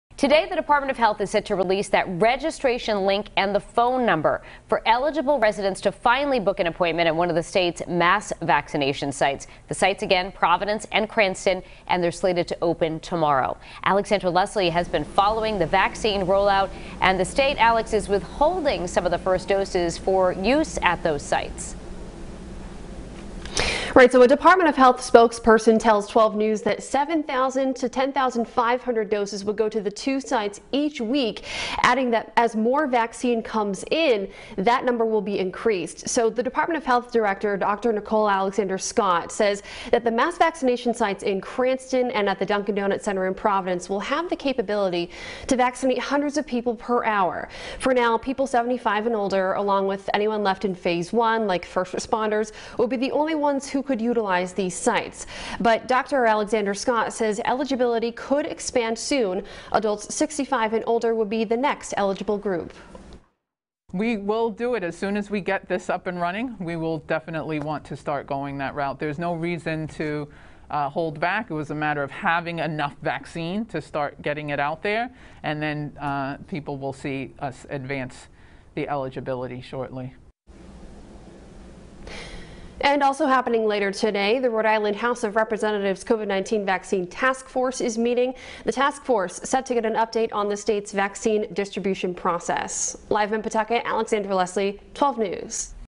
RI to release registration link, hotline to make appointments at state mass vaccination sites
Rhode Islanders in the 75-and-older age range can begin to book appointments Wednesday at two new mass vaccination sites in the state.